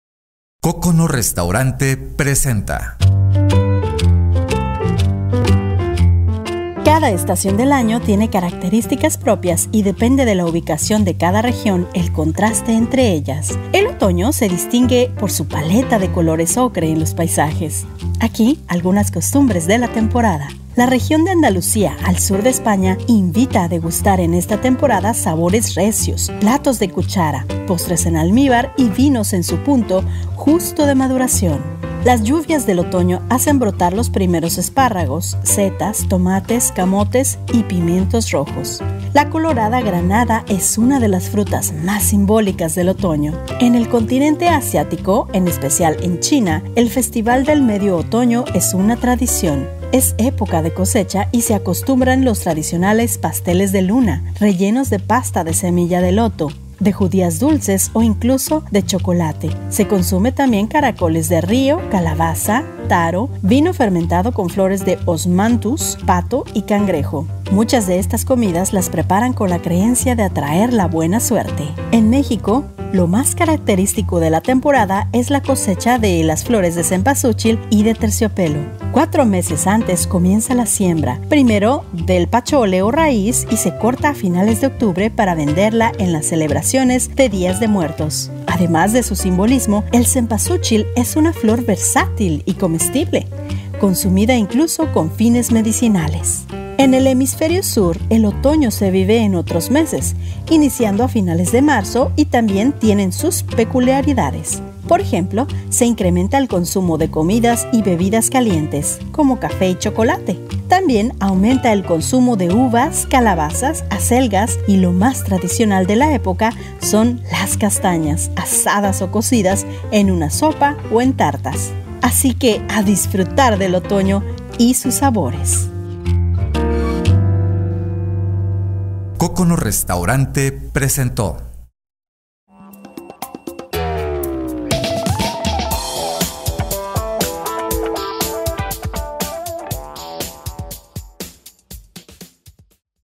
Costumbres culinarias de Otoño | El mundo en un bocado
En esta emisión de Elmundoenunbocado, estamos hablamos de la época de otoño y sus principales productos gastronómicos, aquí te compartimos algunas costumbres de otoño, que al rededor del mundo se han presentado.